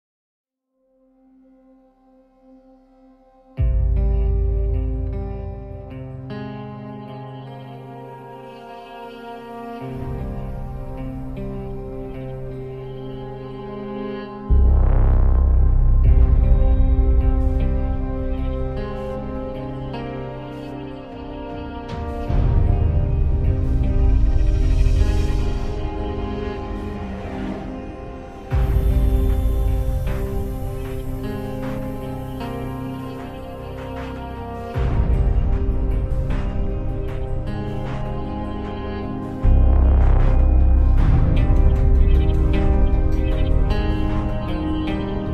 Mannequin Challenge
Momento mannequin challenge durante la espicha que hemos tenido esta tarde noche en la sede y en la que contamos con la visita de Paco Fernández y Rafa Ponzo